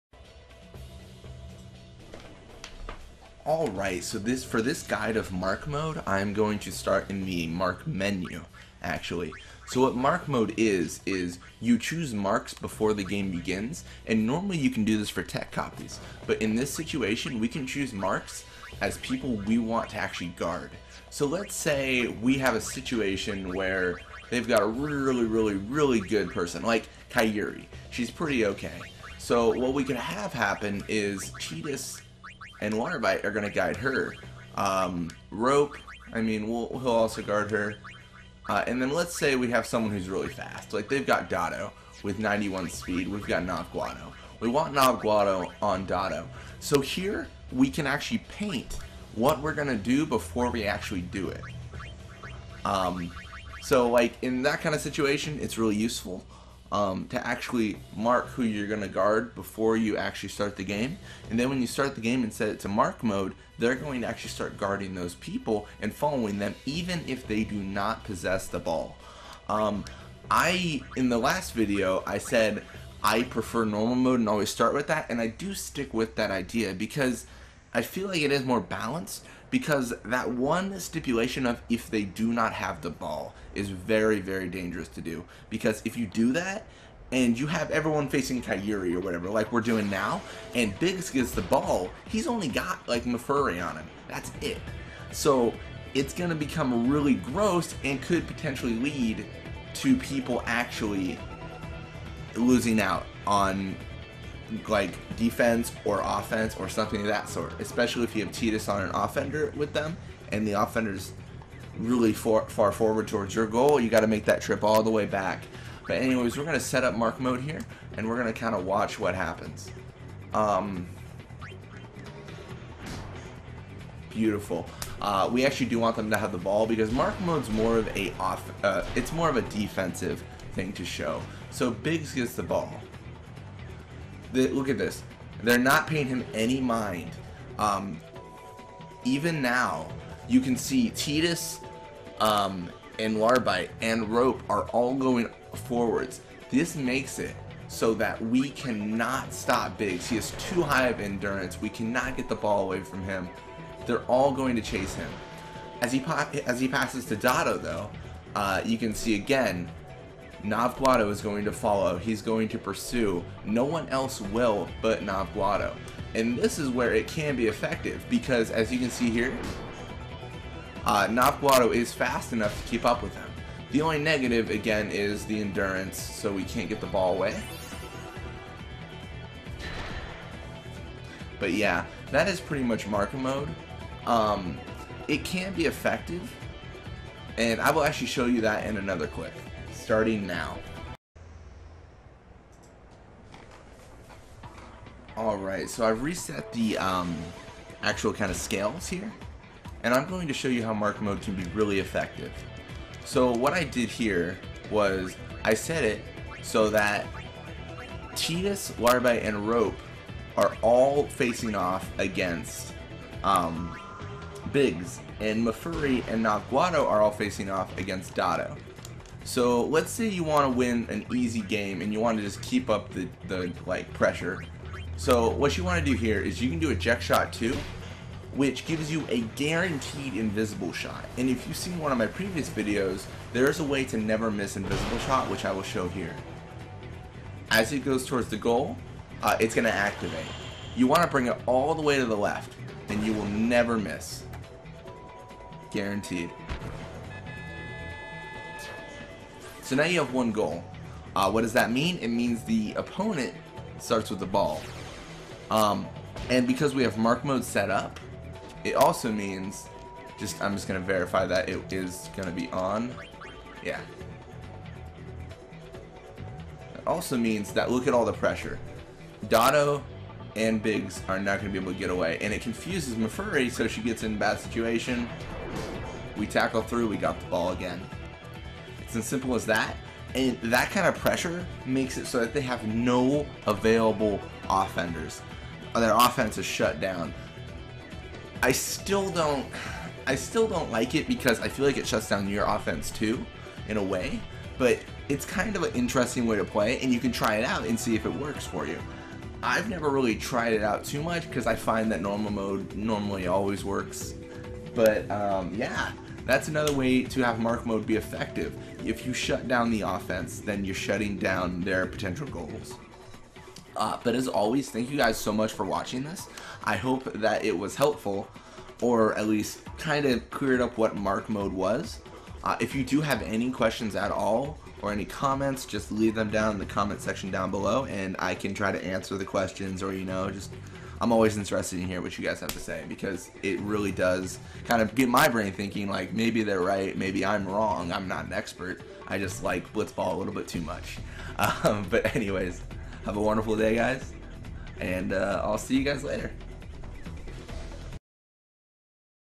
What Is Mark Mode In Blitzball
In this video I define what mark mode is in blitzball and how it can be effective.